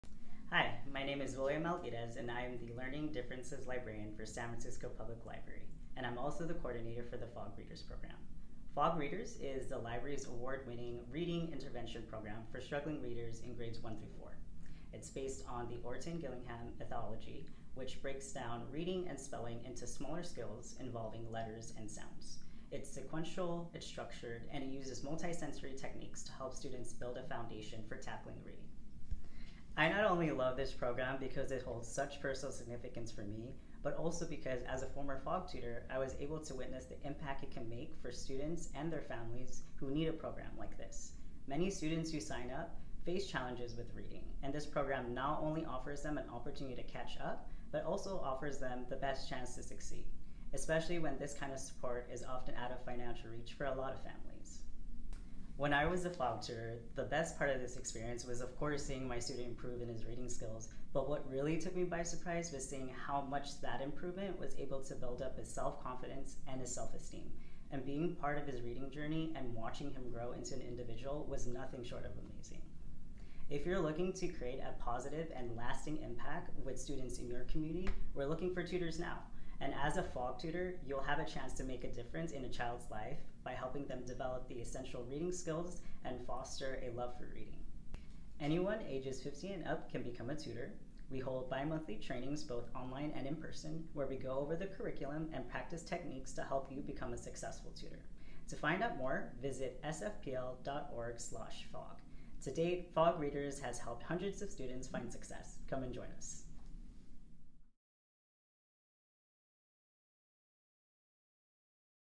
Become a FOG Readers Tutor
Make a positive impact in your community by becoming a FOG Readers volunteer tutor! FOG Readers, an award-winning literacy program, transforms lives. Students read more, gain confidence and improve by over half a grade level in 3 months. Whether you choose to work online or in-person, dedicating just a couple of hours a week can make a big difference for struggling readers in grades 1-4.